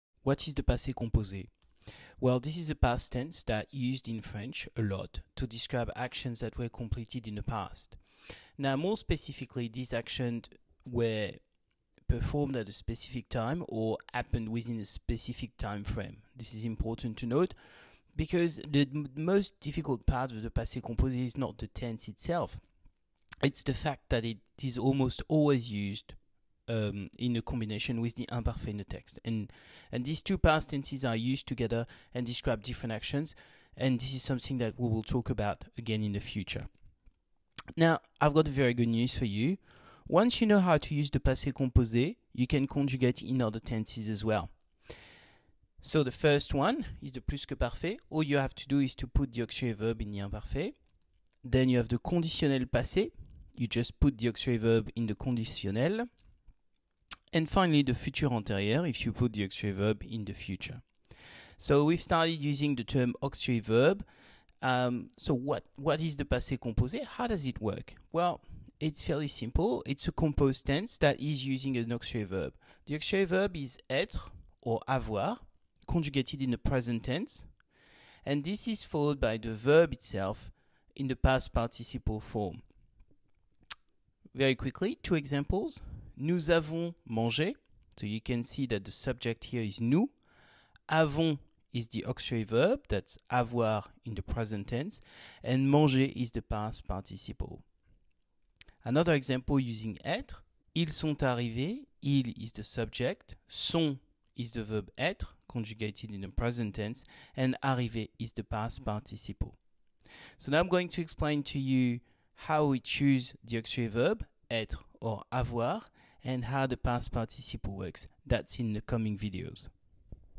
passé composé introduction
An introduction to the passé composé, a very frequently used past tense in French. It i essential to learn how to conjugate in this tense but also to understand when to use the passé composé or the imparfait as both tenses are usually used simultaneously to talk about past events.